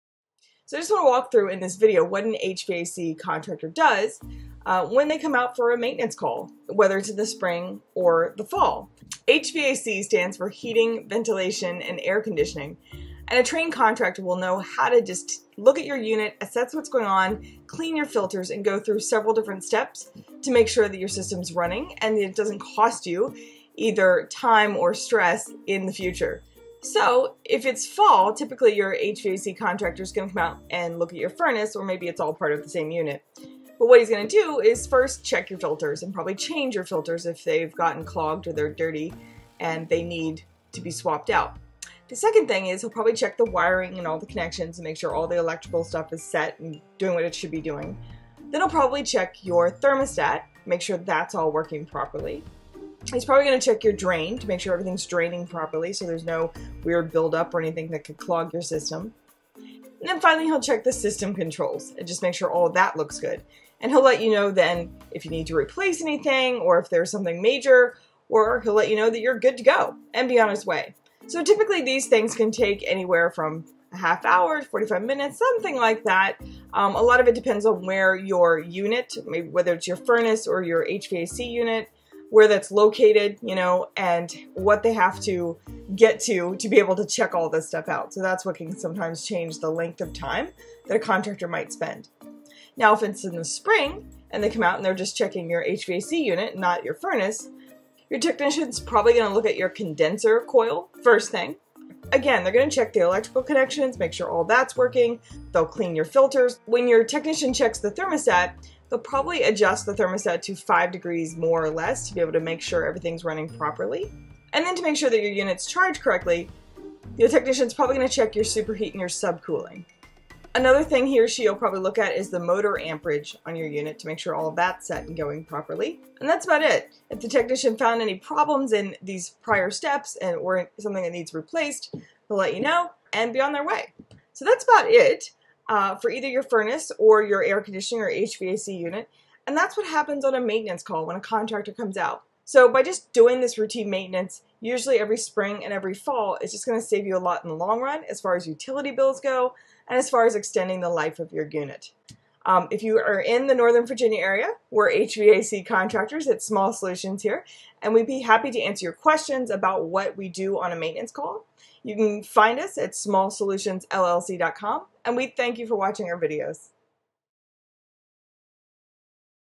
Small Solutions HVAC Services
What does an HVAC technician do when they make a maintenance visit to your home or office?

HVAC technicians install and repair industrial and commercial refrigerating systems. But a lot of what we do at Small Solutions is regular maintenance. We observe and test system operations, using gauges and instruments, to test the health of your system and find anything that might need replaced to keep things operating efficiently.

Things we do during a maintenance visit include:

• Test lines, components, and connections for leaks
• Adjust valves according to specifications and charge system with proper type of refrigerant by pumping the specified gas or fluid into the system
• Adjust or replace worn or defective mechanisms and parts
• Perform mechanical overhauls and refrigerant reclaiming
• Review the electrical connections and thermostat performance
• Ensure your system is set for the upcoming season to run efficiently
• Watch a quick overview of what we check during a spring or fall maintenance visit: